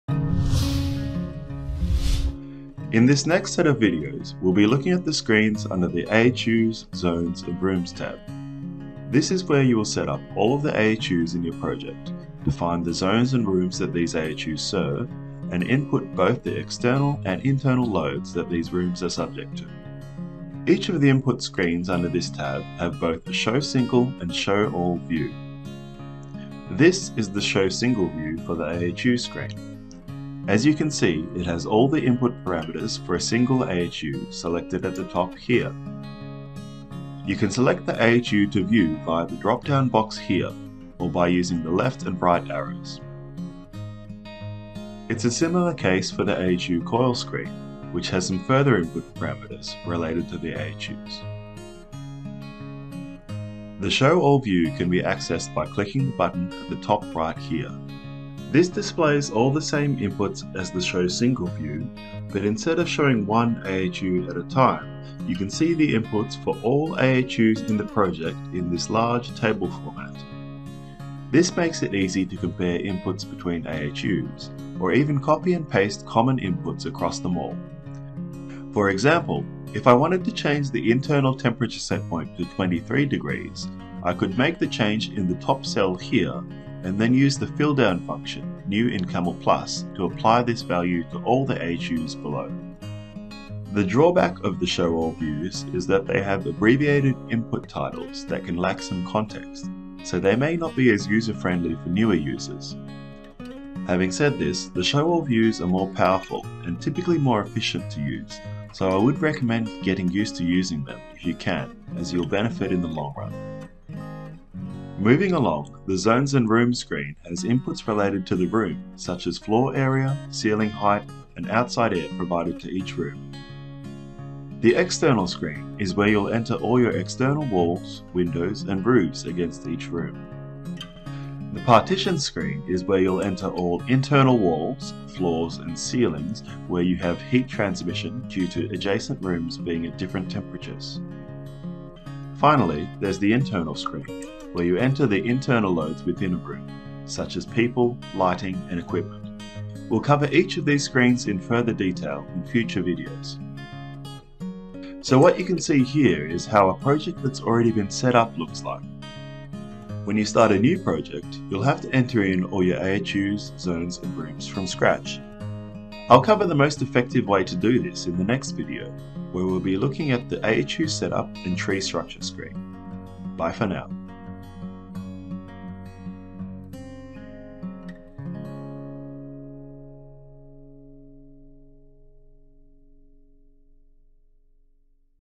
CAMEL+ Basics 11: AHUs Zones and Rooms Overview
This tutorial provides a high level overview of the CAMEL+ AHUs Zones and Rooms input screens and Show All vs Show Single views.